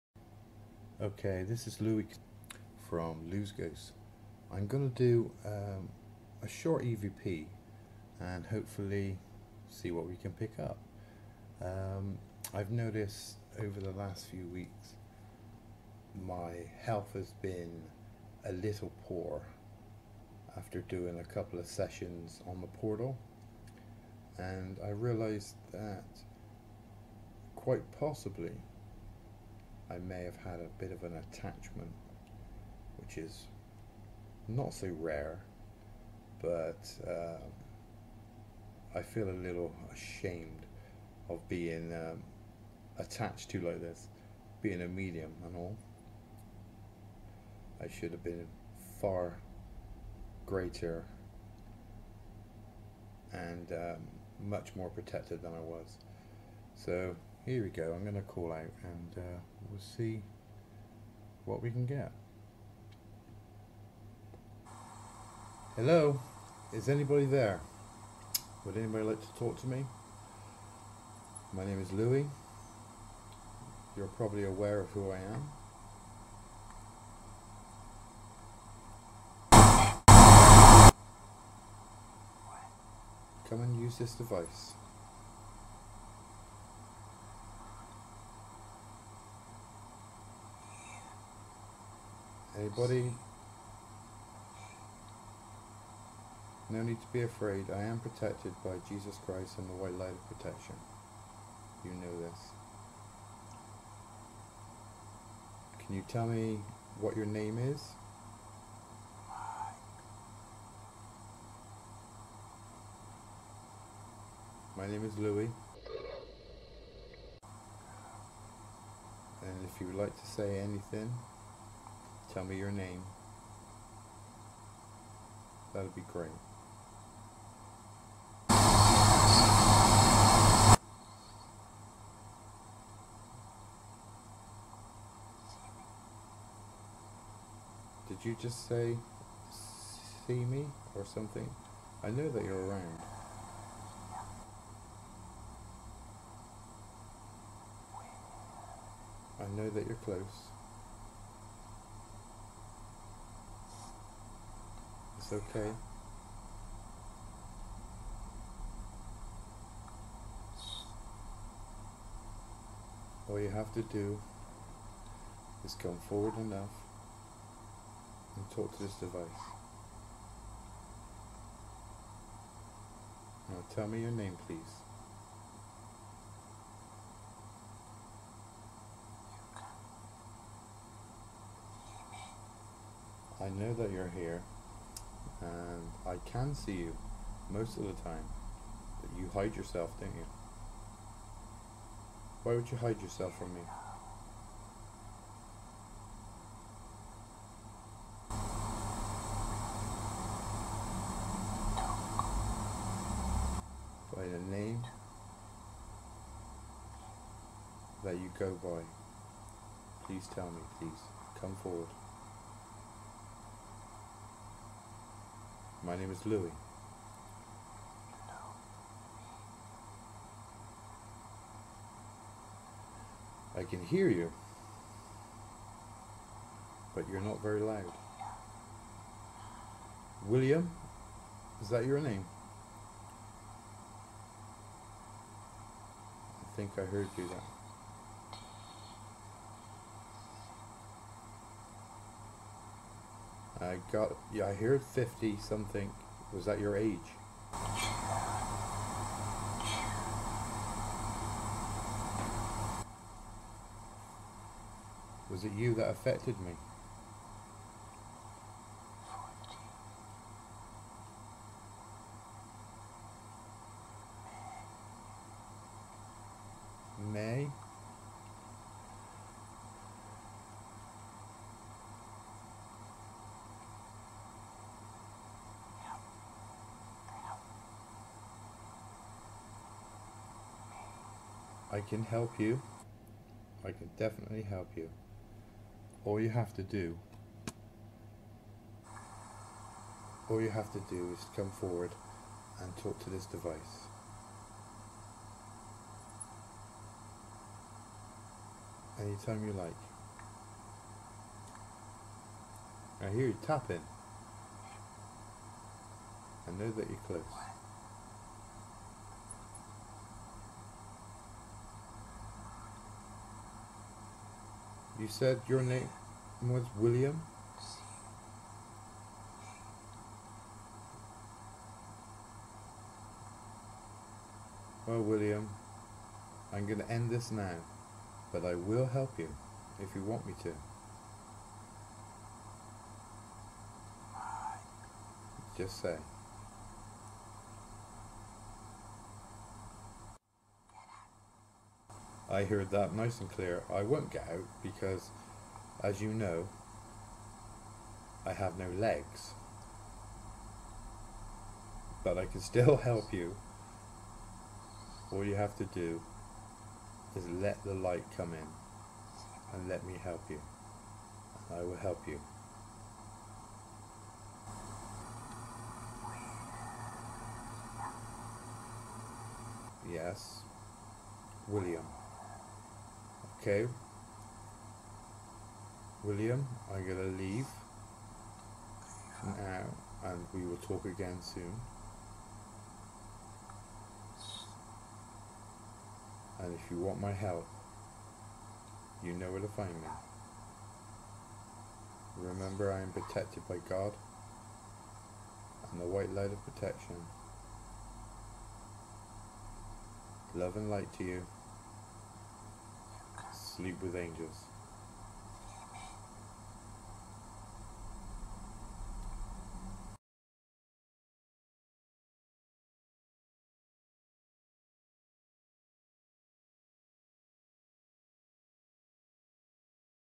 Evp1
A short evp using two recording devices and white and pink noise in the background, I did this after having a spirit attachment leaving me very unwell for a month,so figured I would ask who it was and there was two names Mike and William only a short video but did get a few things headphones on as it's quiet thanks :)